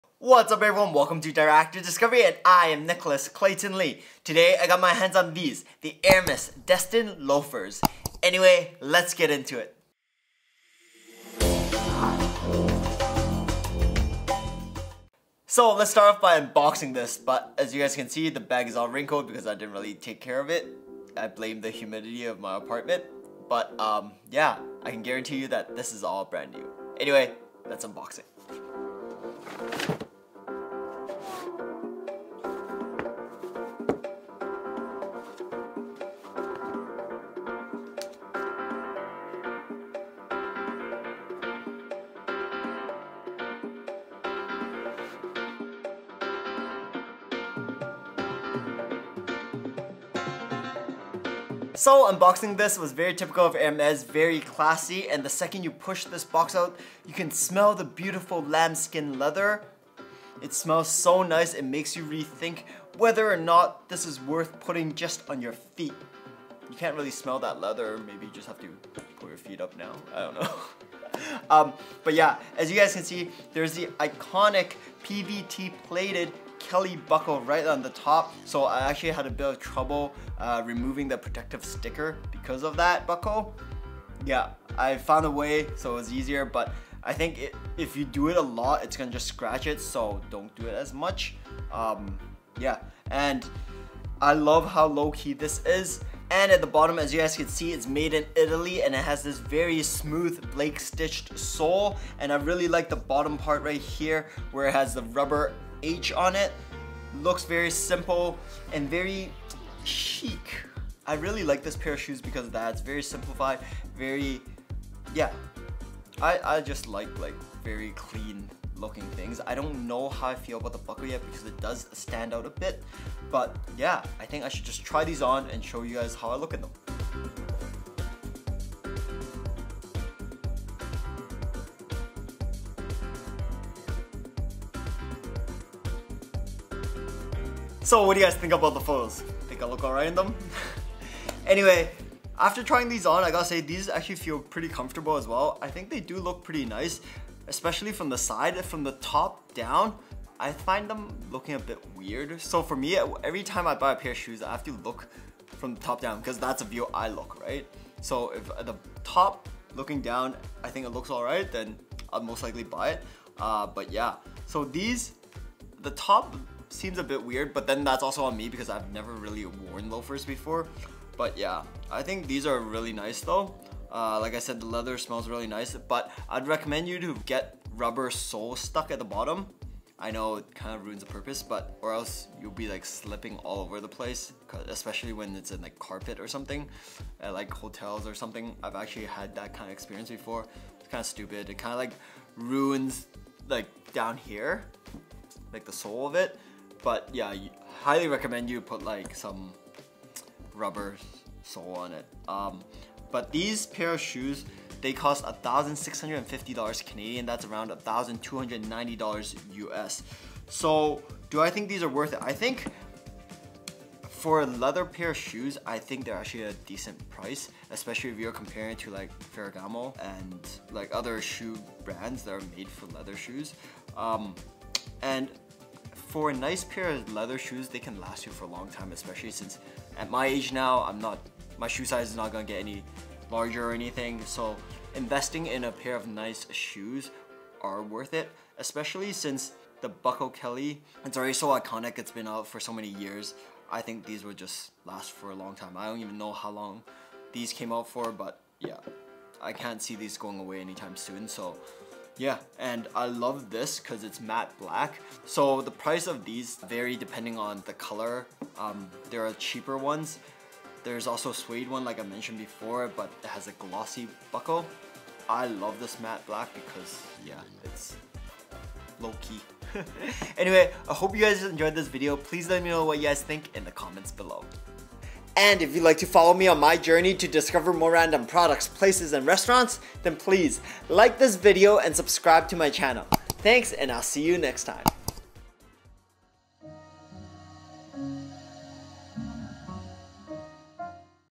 Hermès Destin Loafers UNBOXING and QUICK REVIEW!
Unboxing and reviewing these Hermès Destin Loafers for men!
Gear
Camera: Fujifilm X-T4
Lens: Fujifilm Fujinon Prime Lens XF35mm F2 R WR
           Fujifilm Fujinon Zoom Lens XF18-55mm F2.8-4.0 LM OIS
Audio: Rode VideoMic NTG
Light: Godox SL-60W CRI 95
Tripod: Benro A2573FS4 Single Leg Video Tripod Kit
Others: Reflector Holder